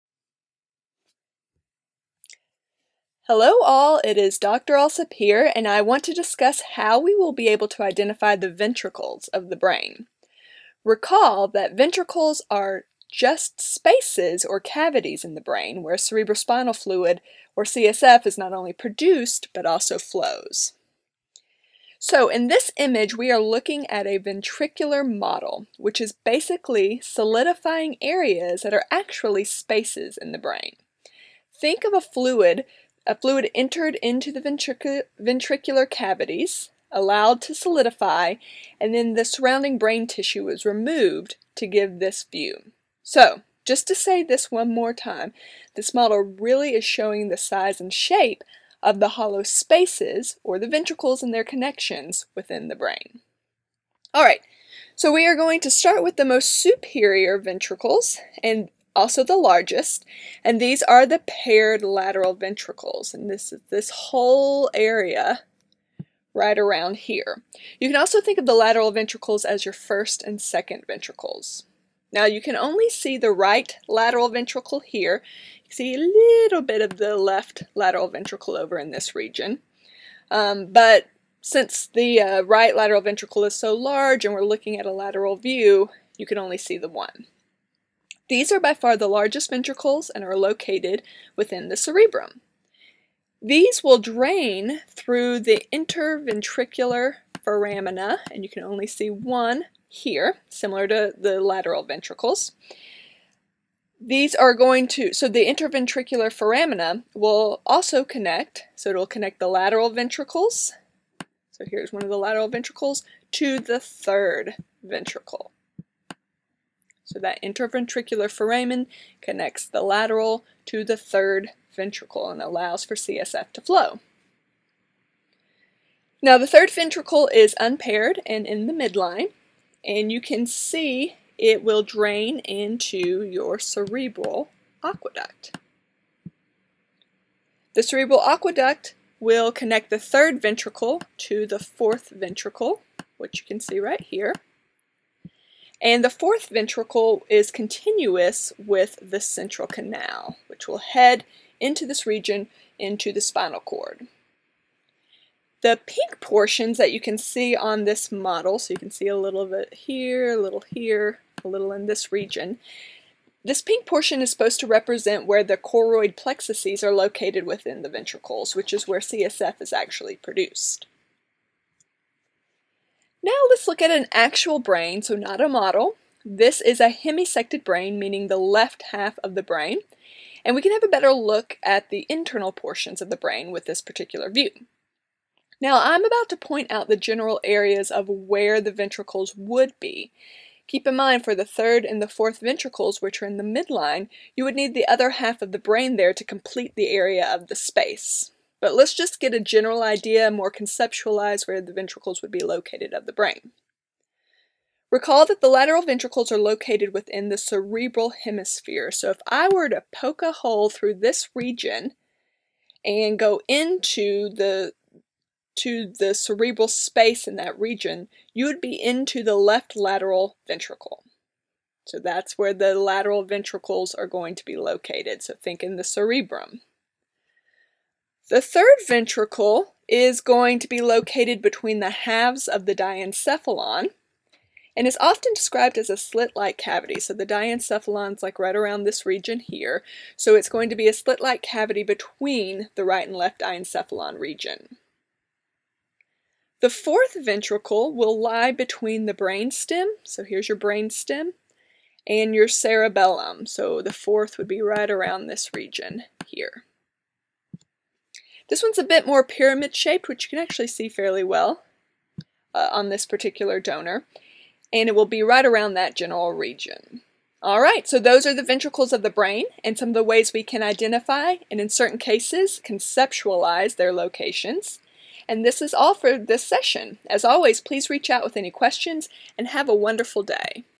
Cranial Cavity and Brain - Ventricles
Video describing tips to best identify the ventricles associated with the cranial cavity and brain in relation to the supplemental dissection videos for the DENT 545 Dental Head and Neck Anatomy course at the University of Michigan.